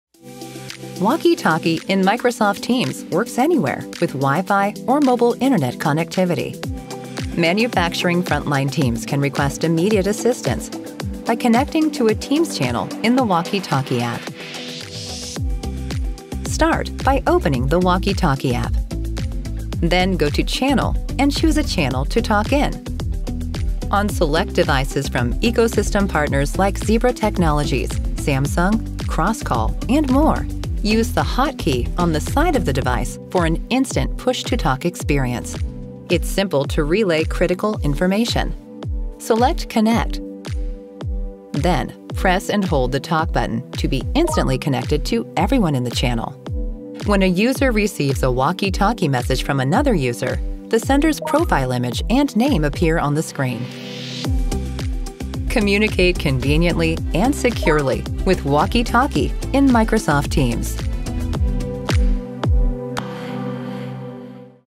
Using Walkie Talkie in Microsoft Teams
Communicate easily and securely with Walkie Talkie in Microsoft Teams.